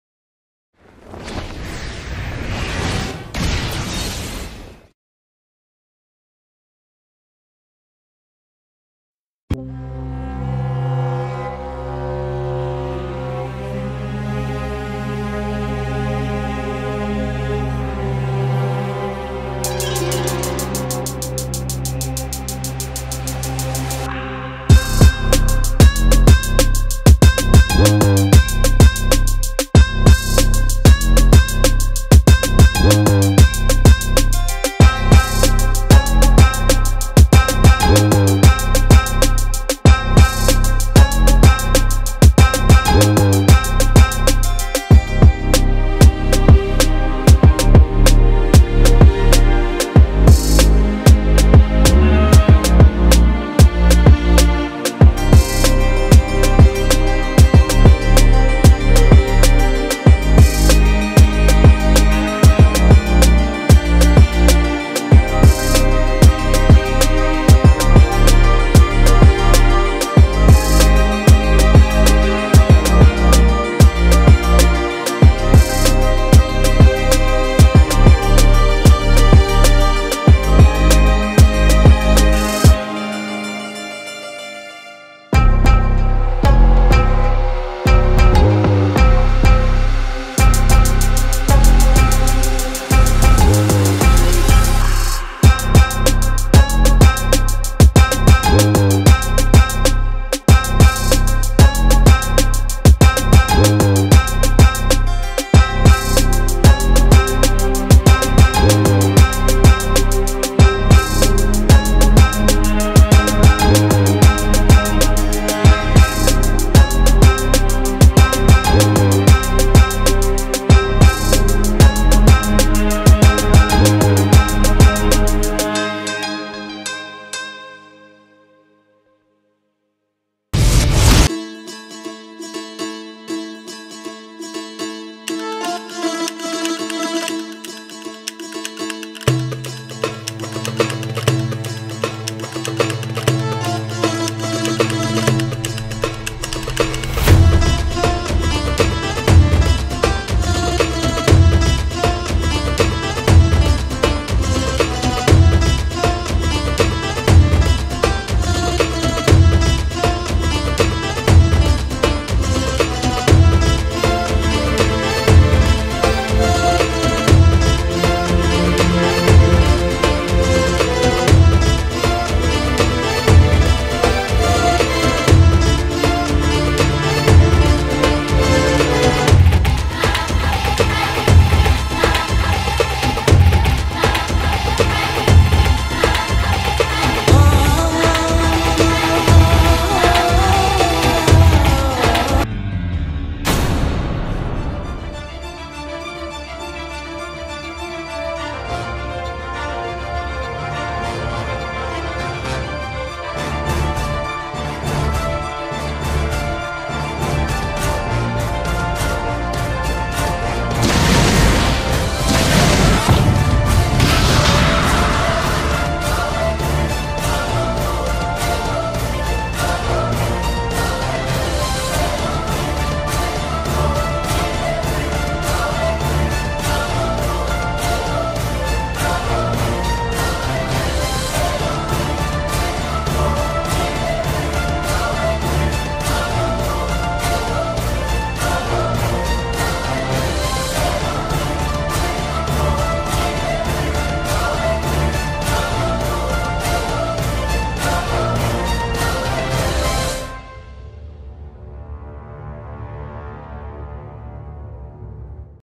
Chou vs Badang | Mobile Legends Bang Bang | Flipaclip
This animation took me 2 months to finish. Hope you Like it! I made this on my phone with Flipaclip.